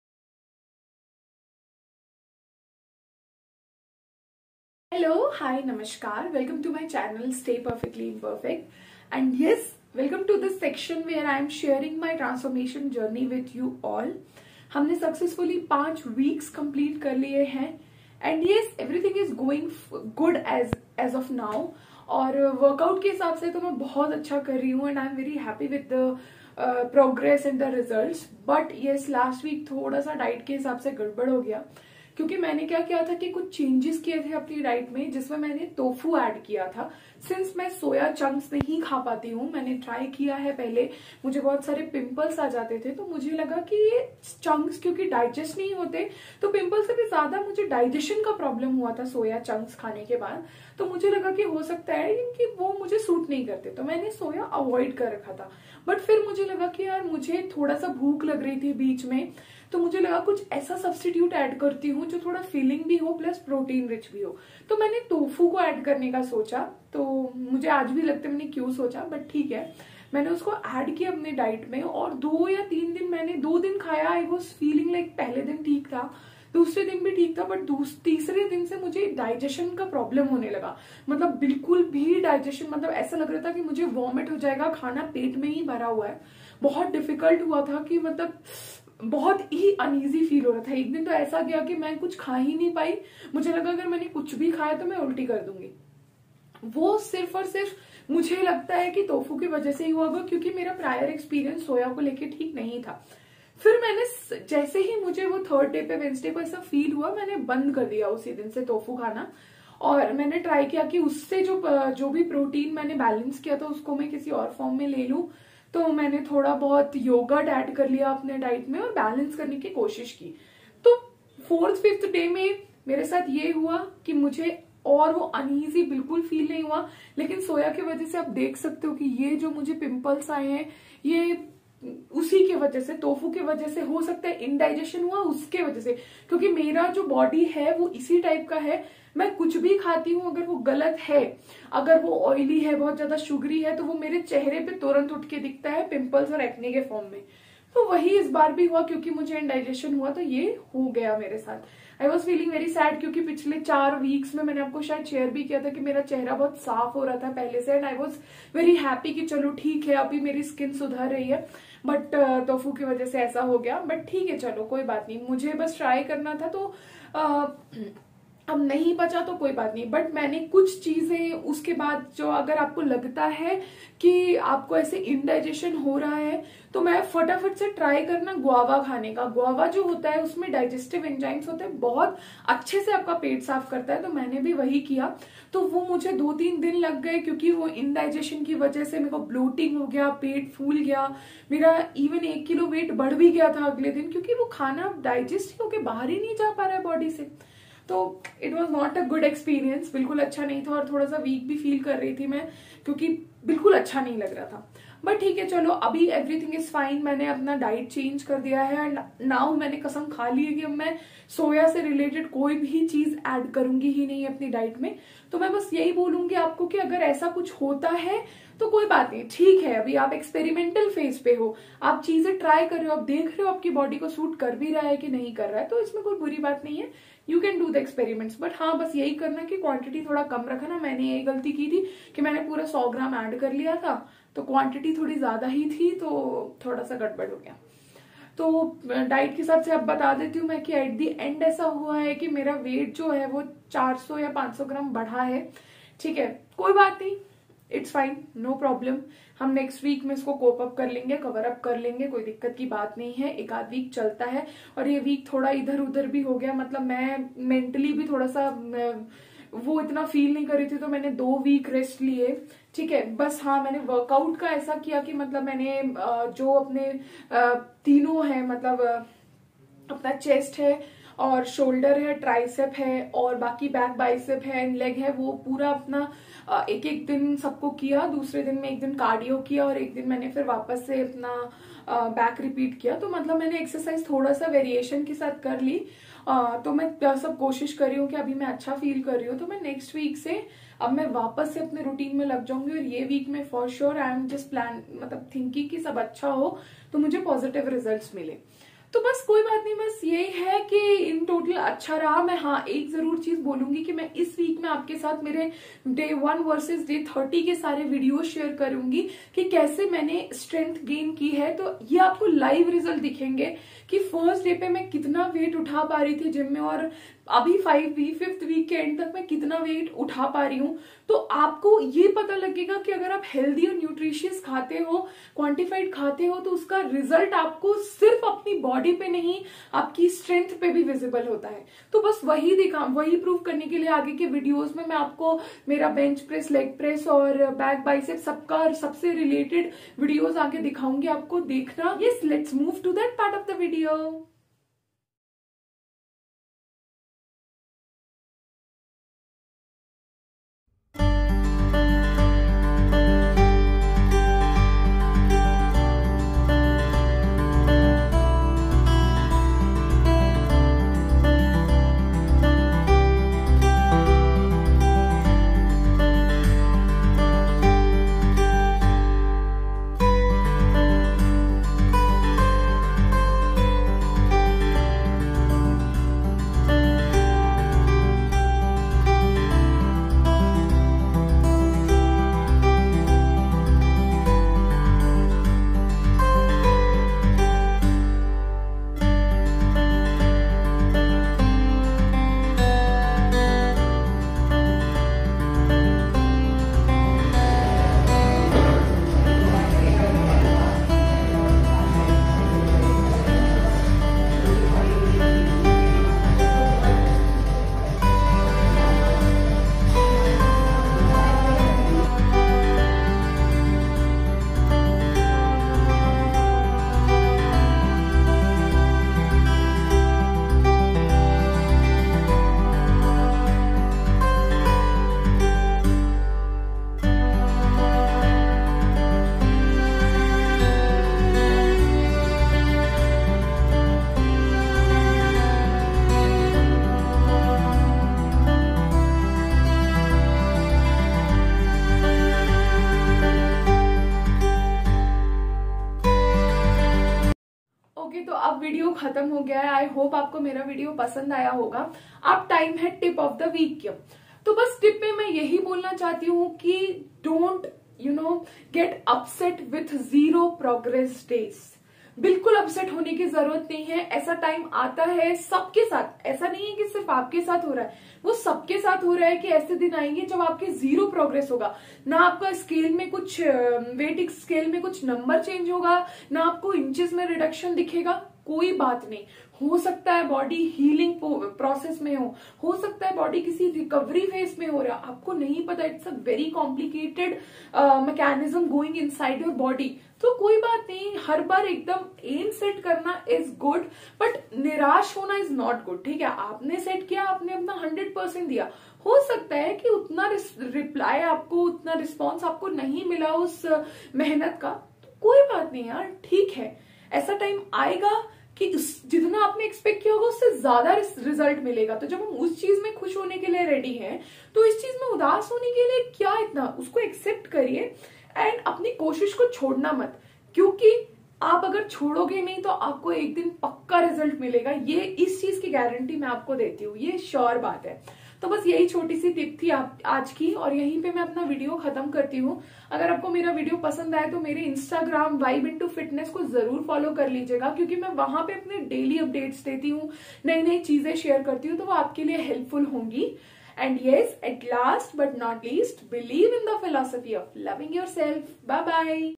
My 12 Weeks Transformation Journey| Week -5 Vlog| Diet, Workout and Daily Updates of Week -5
Hello Dosto,